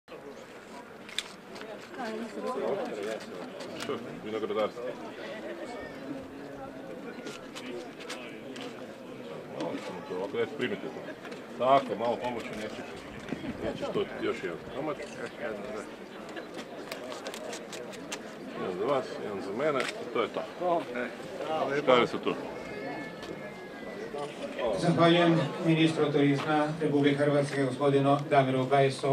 Sveti Vinko rezanje vrpce ministra Bajsa kod otvorenja kipa svetog Vinka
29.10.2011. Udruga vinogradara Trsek
⭐️ Pratite nas!